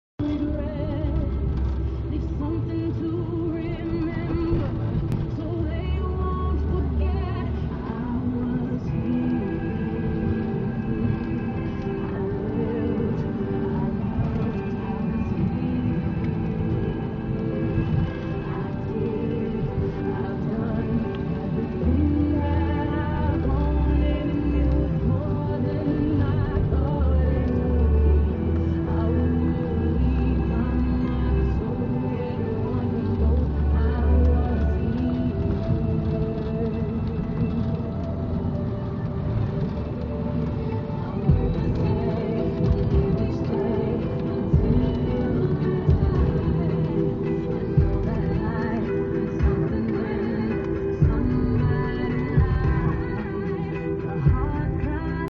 The Rain Memory
A video in which I was in a car filming from my phone. Thinking mode...